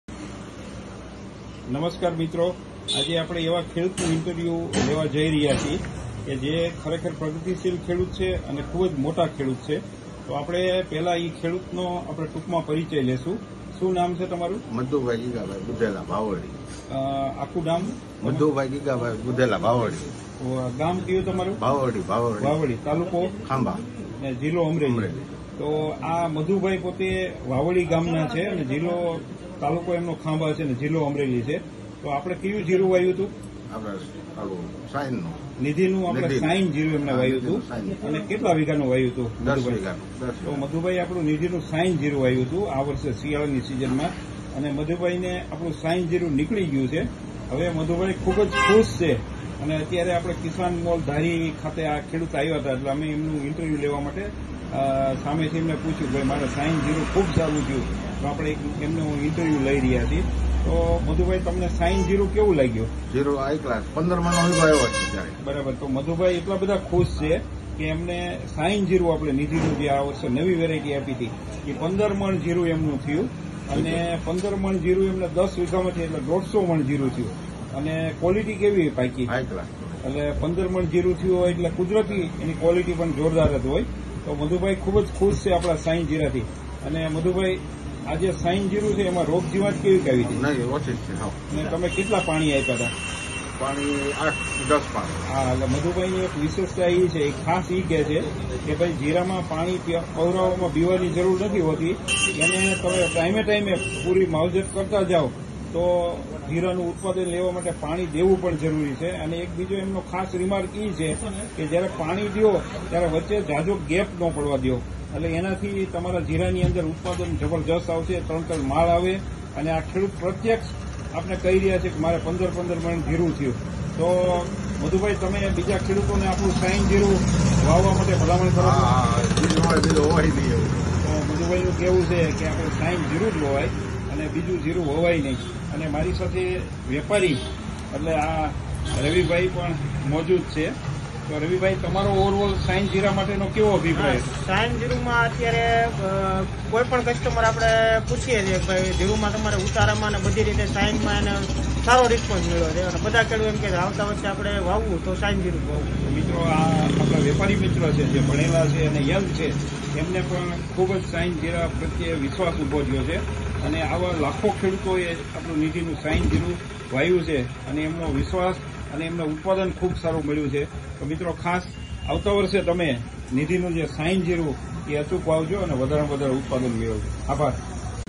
જીરા બીજ મા નંબર ૧ બ્રાન્ડ - નિધી શાઈન
પ્રસ્તુત છે શ્રેષ્ઠ જીરા બીજ - નિધી શાઈન
ખેડૂતમિત્રો, વાવેતર કરો નિધી શાઈન ને મેળવો મબલખ ઉત્પાદન .

• નવું સંશોધિત જીરા બીજ
• સૂકારા સામે વધુ પ્રતિકારક જાત.
• પેટા ડાળી ની સંખ્યા વધારે.

વધુ માહિતી માટે અમારો સંપર્ક કરો : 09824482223, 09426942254